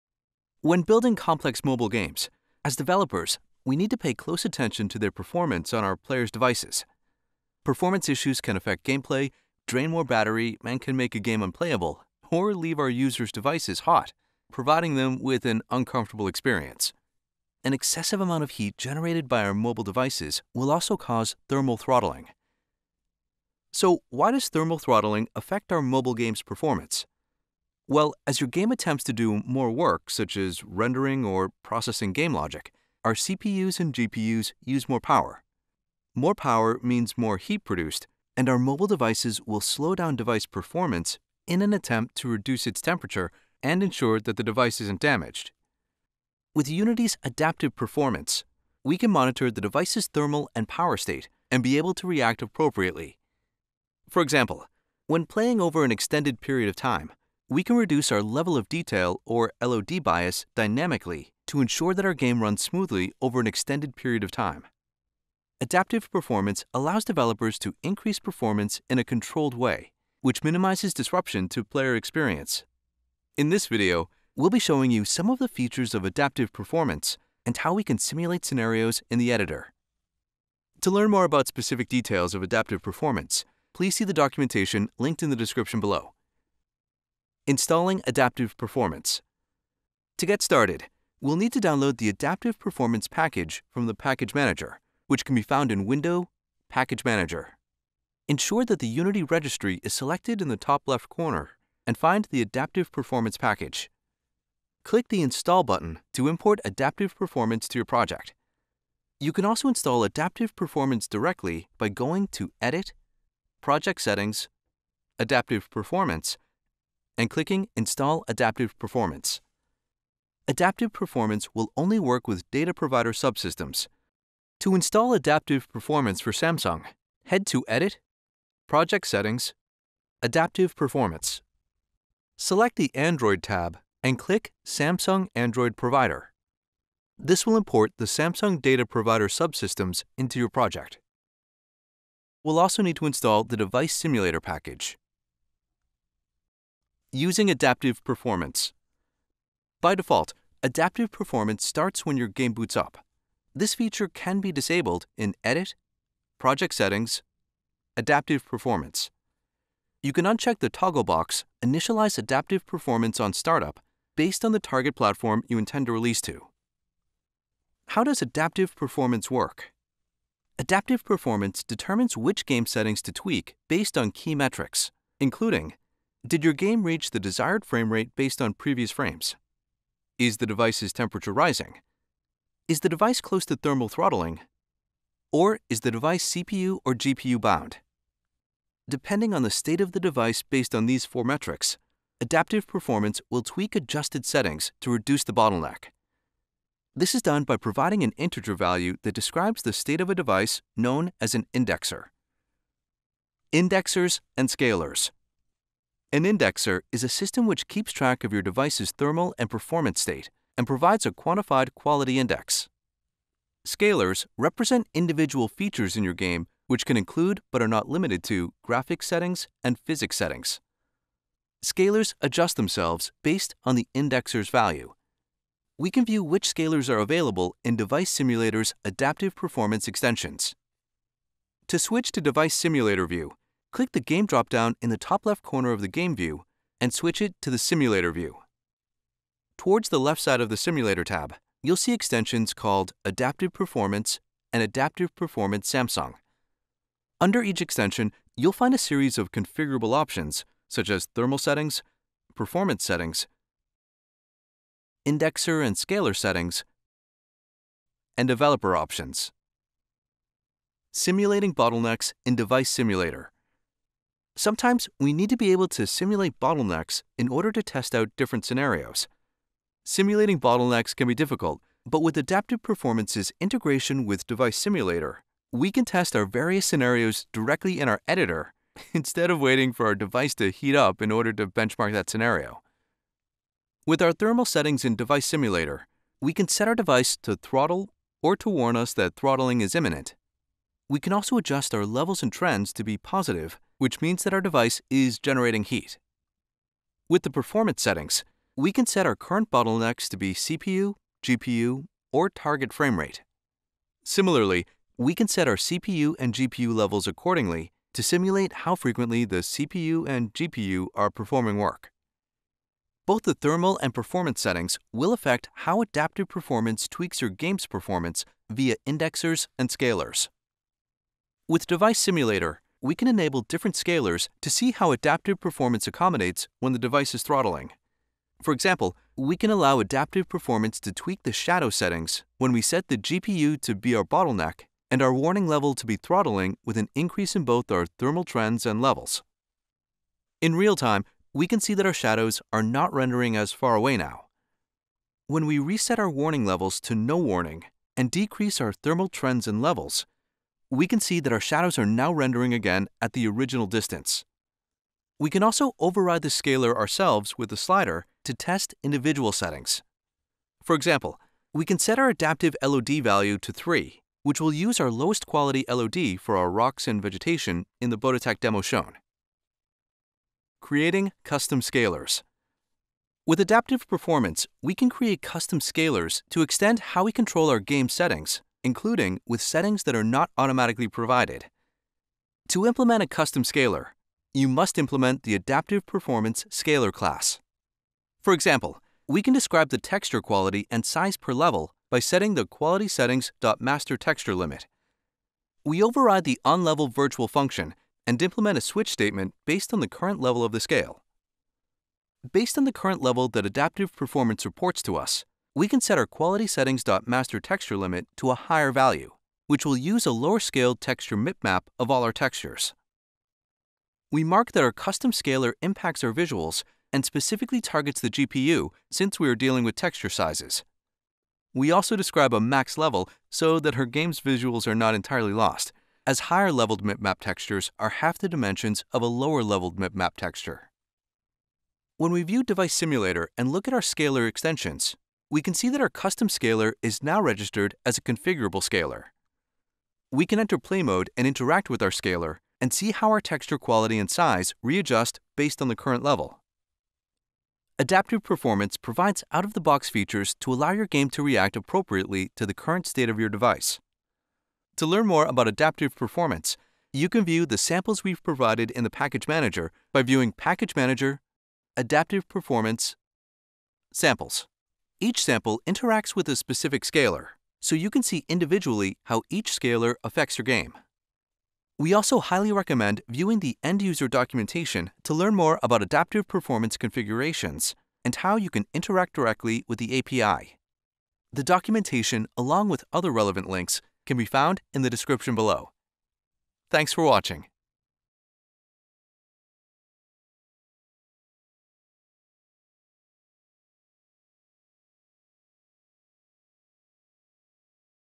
Build stunning mobile games that run smoothly with Adaptive Performance
Optimize your mobile game for Samsung devices by using Adaptive Performance. Discover how it checks the device’s thermal status and adjusts the target frame rate, level of detail, and CPU/GPU clock to increase your game’s performance quality.